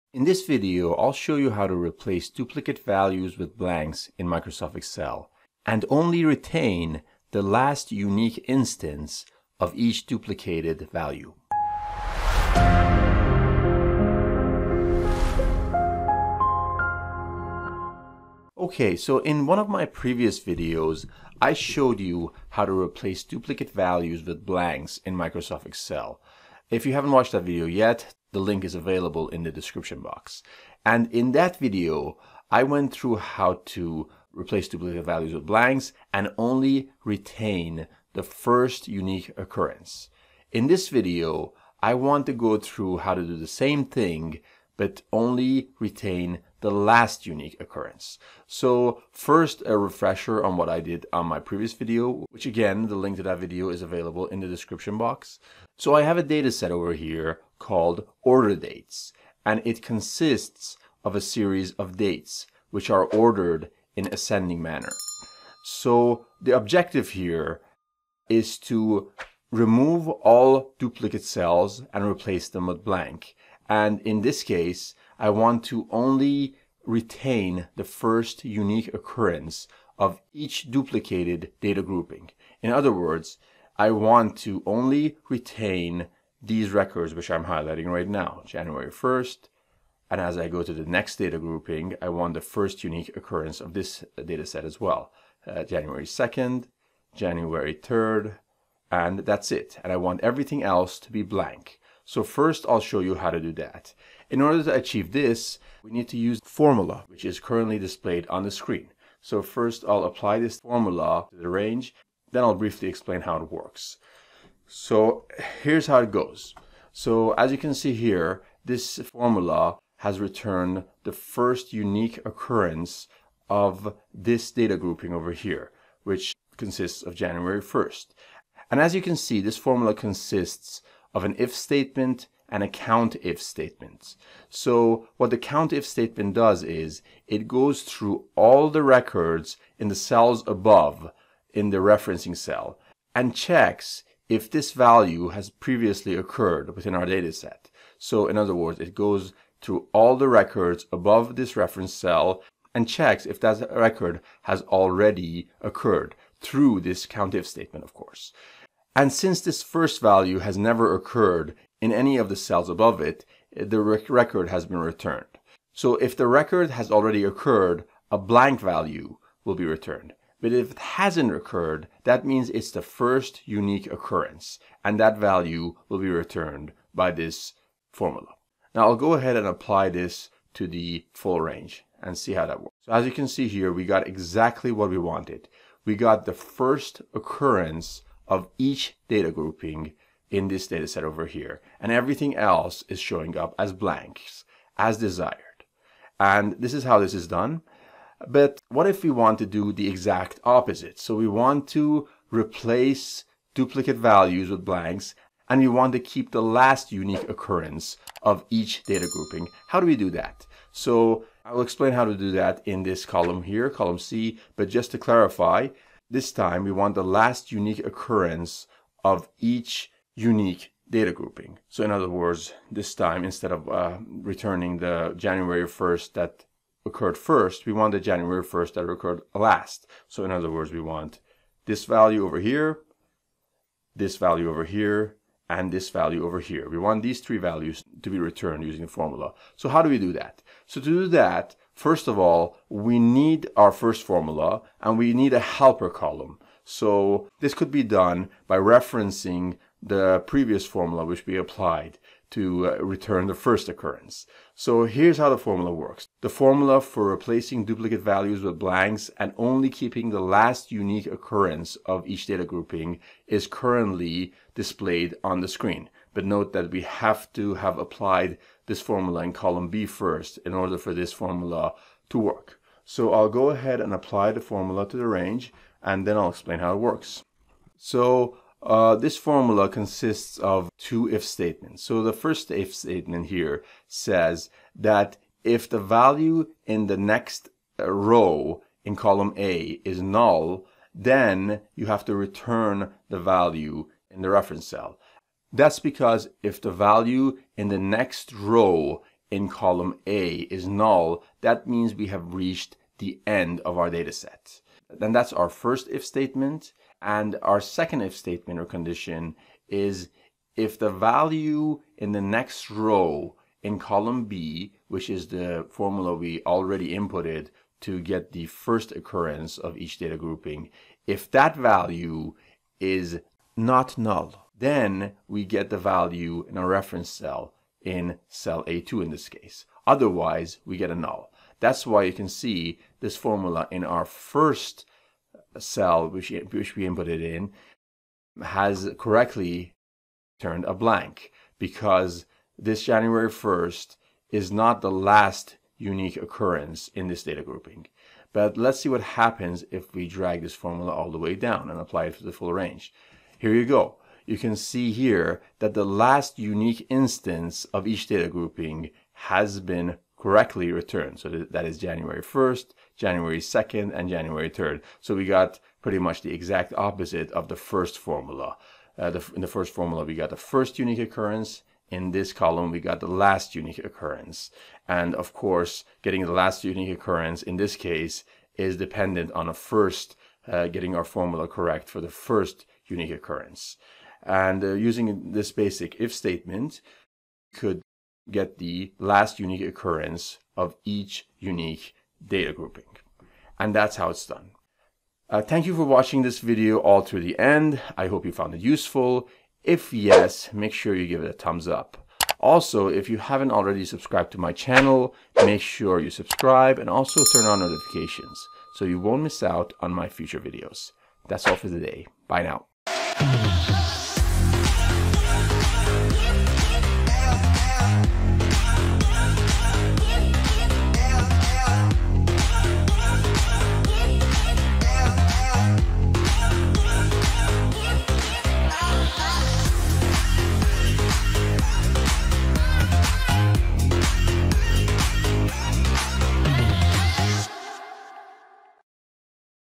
HOW TO REPLACE DUPLICATE VALUES WITH BLANKS IN MS EXCEL - Only Retain Last Unique Occurrences
In this video you will learn How to Replace Duplicate Values with Blanks in MS Excel & Only Retain Last Unique Occurrence.

Link to my previous video on "How to Replace Duplicate Values with Blanks in MS Excel"